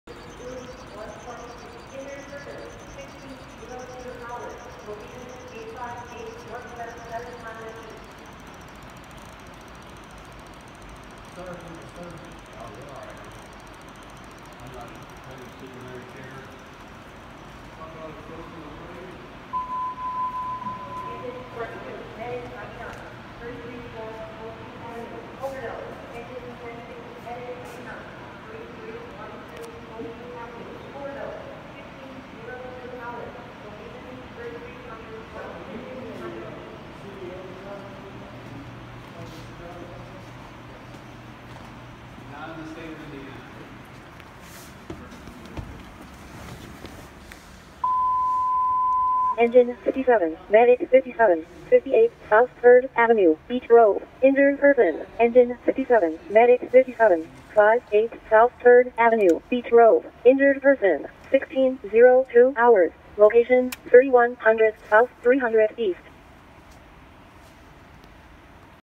Indianapolis reserve Squad 7 demo of lights 01-19-21
We went back to Indianapolis to visit with the crews and sadly to hear the brand new squad they had last year was involved in a traffic accident and totaled it so now they are using a reserve truck which is seen in the third picture. ￼